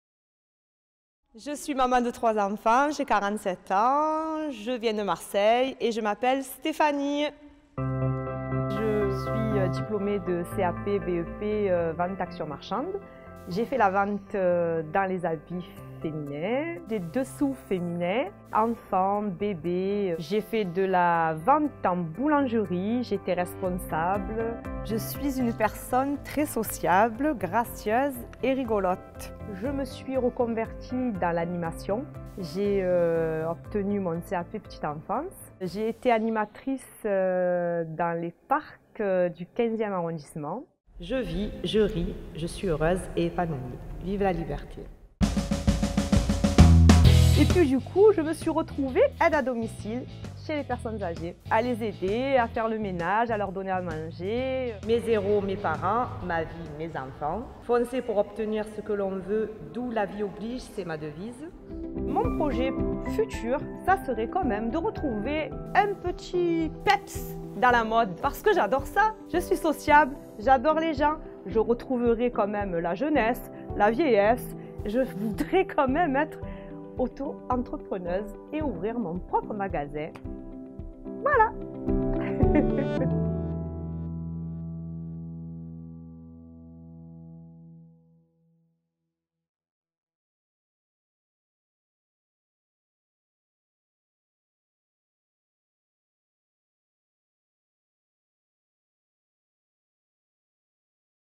"Ça tourne pour moi !" Le CV vidéo de Stéphanie.
Dir. artistique Lauréline Kuntz, réal. et montage Grégoire de Castelbajac.
A Marseille, le projet "Ça tourne pour moi !" accompagne des personnes dans leurs demandes d’emploi, Il propose aux participants d’investir un processus artistique qui associe la pratique du cirque et la réalisation de CV vidéo. En co-construction avec l'agence Pôle emploi Cap Pinède, ARCHAOS-Pôle National Cirque et les artistes de l'école de L’Opéra de la parole, le projet.

La pratique du cirque est un vecteur fort d’estime de soi, de vivre ensemble, de lutte contre l’exclusion, de goût de l’effort et d’esprit d’équipe. L’intention fondatrice du projet, identifiée par l’équipe de l’agence Pôle emploi-Cap Pinède et Archaos-Pôle National Cirque, est la remobilisation des personnes en recherche d’emploi à travers une expérience artistique corporelle et l’expression de soi. Au-delà de l’apprentissage d’une technique, ces ateliers ont pour vertus d’encourager le dépassement de soi, l’entraide, la conscience corporelle et le lâcher prise.

Suite à ces temps de pratique prennent place les ateliers de réalisation de CV vidéo, menés par les intervenants de l’école de l’Opéra de la parole. Ces moments se déroulent en quatre phases : écriture, mise en scène, tournage et montage. Ils s’appuient sur les apports des ateliers de pratique pour travailler l’éloquence, la confiance en soi et en son parcours, la présentation de soi et le travail collectif.

Afin de valoriser le travail des participants, deux soirées de projection des CV vidéo et d’un reportage sur le projet intègrent la programmation de L’Entre-Deux Biennales 2022.

En parallèle de ces étapes de travail, Archaos propose aux participant.es d’assister à plusieurs Rendez-vous cirque dans le but de rencontrer ces artistes en création et d’échanger avec eux.elles autour du processus de recherche.